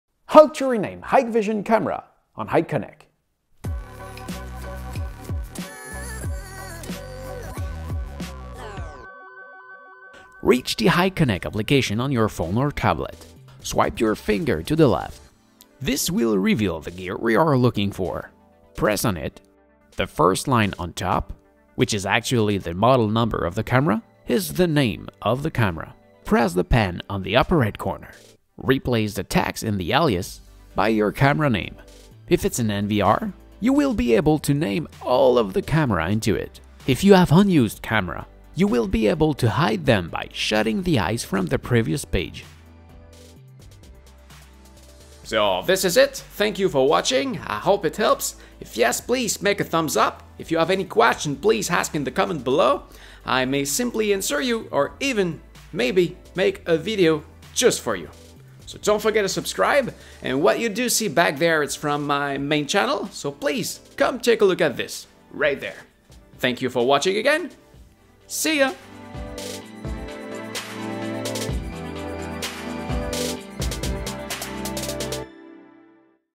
How to rename Hikvision Camera on Hik-Connect
Having the model number of a camera as name is not very reliable when you are looking for a specific camera.
Let me show you how to rename them on the Hikconnect application.